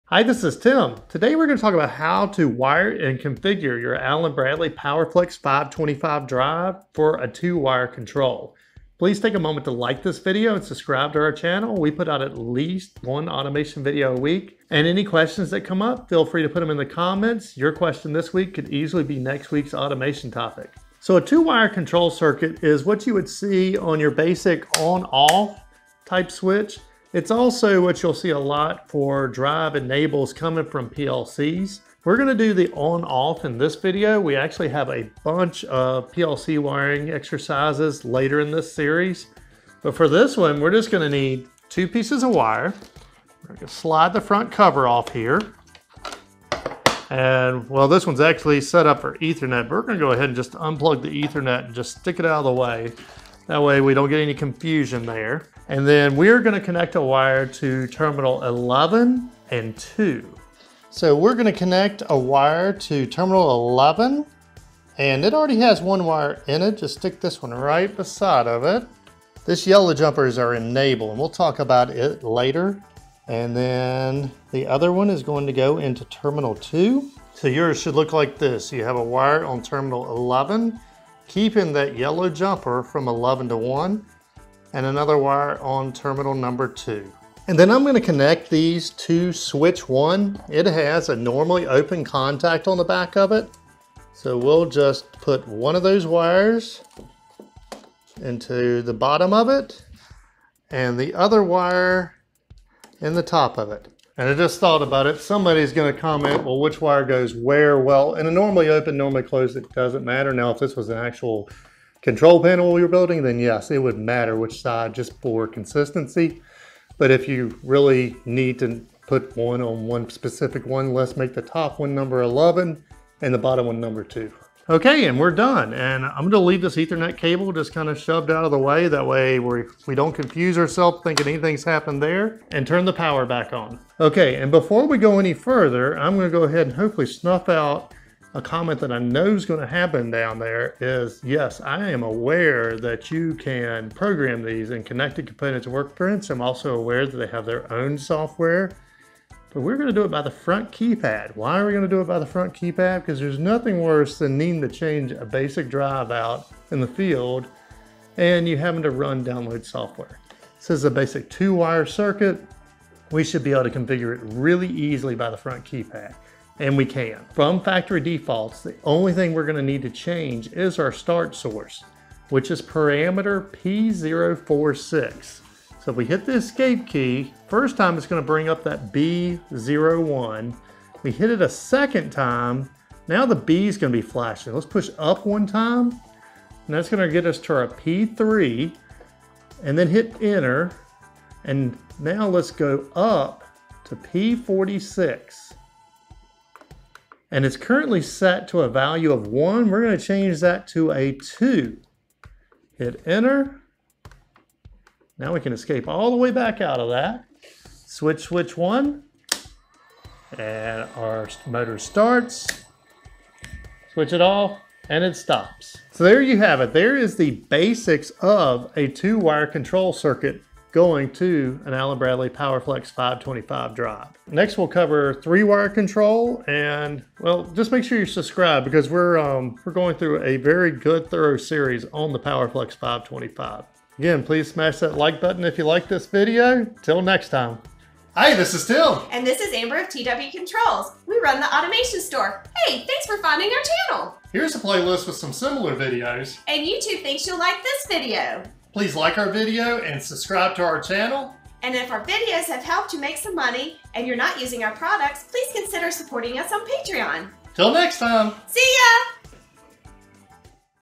How to Wire a Powerflex 525 for a 2 Wire Contact Closure
In this video we show you how to wire and configure your Allen Bradley Powerflex 525 AC Drive for 2 Wire Control.